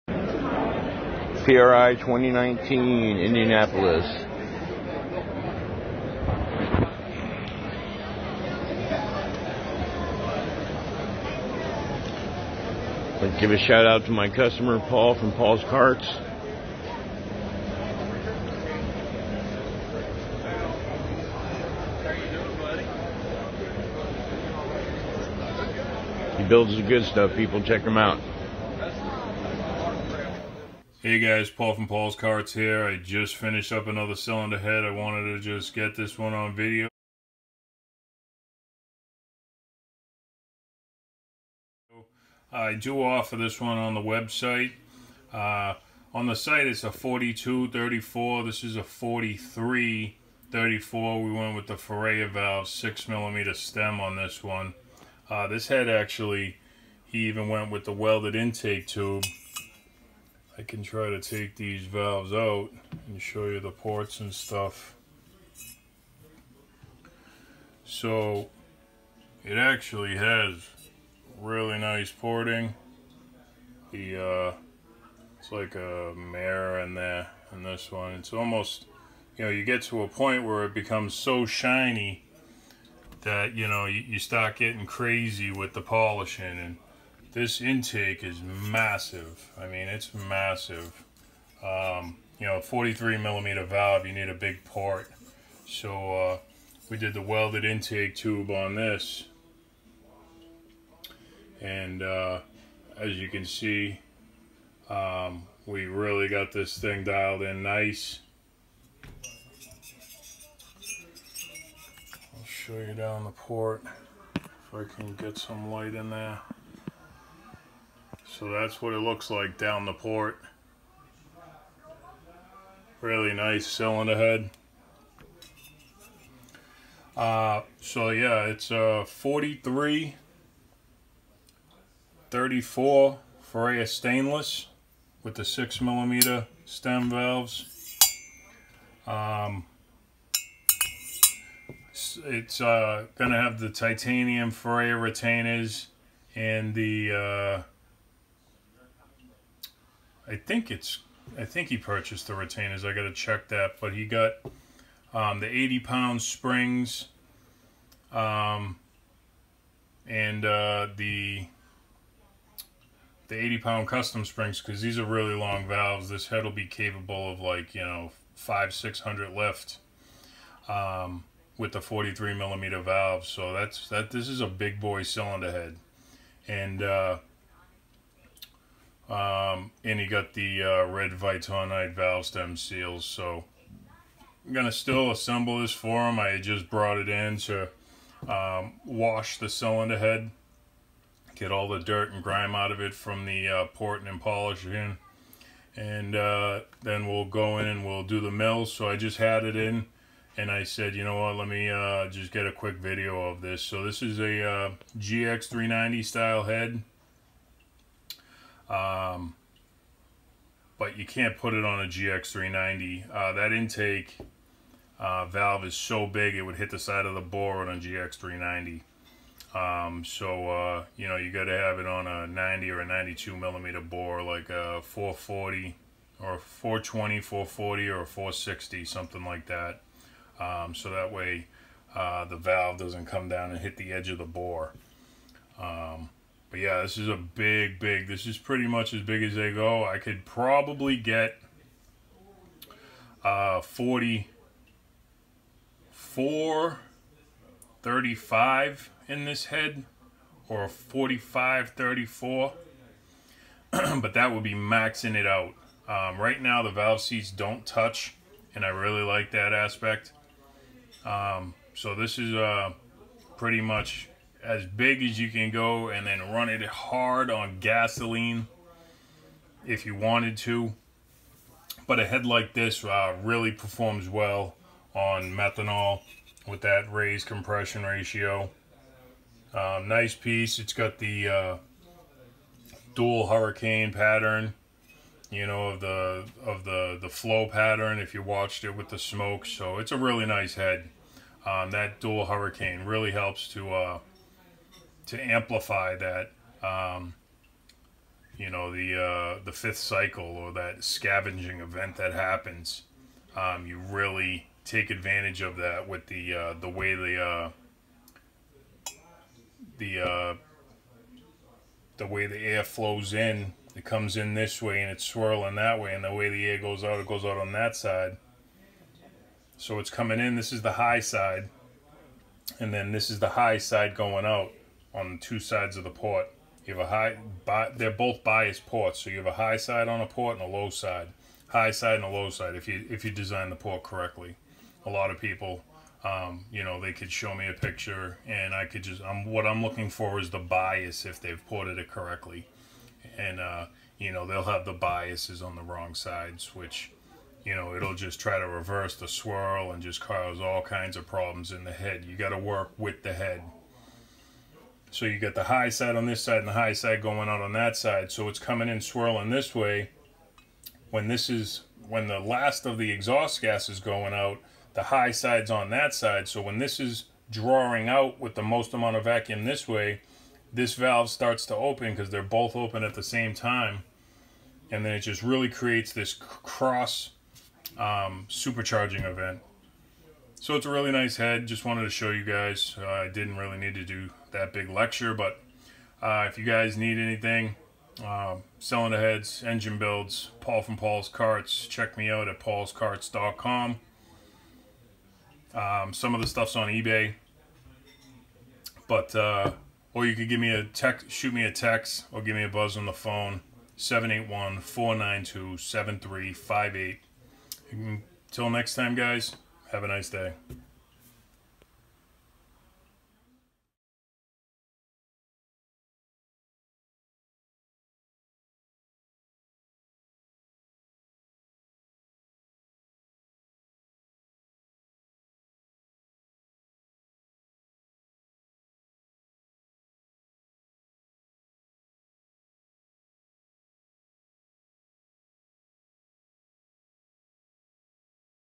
JD from SI gives a shout out! And Big block GX390 welded 43/34 head
This is billy’s head big block gx460 head with 43/34 Ferrea 6mm stem valves and welded dual hurricane head with welded intake tube “38mm ID !!”

Paulskarts.com